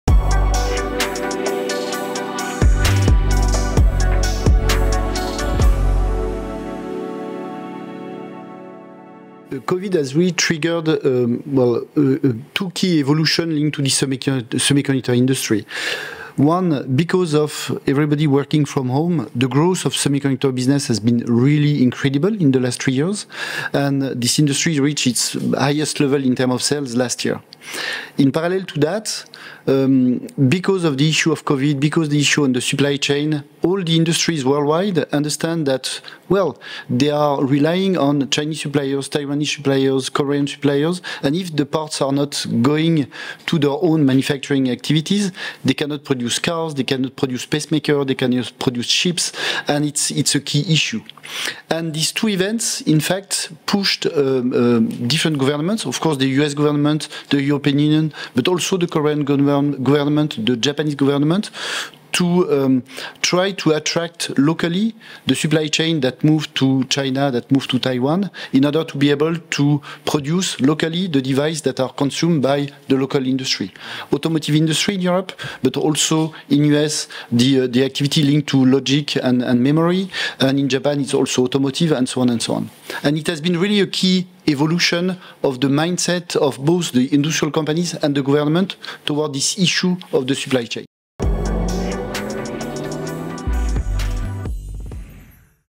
YOLE Jean Christophe Eloy - Devlopment of semiconductor industry last year
Jean Christophe Eloy CEO Yole Developpement What developments have occured in the semiconductor industry over the past year? "Global mindset evolution of both industrials and governements towards a local relocation of the supply chains."  To explore more about devices, reshore, relocation, covid, supply chain, industrial production, , feel free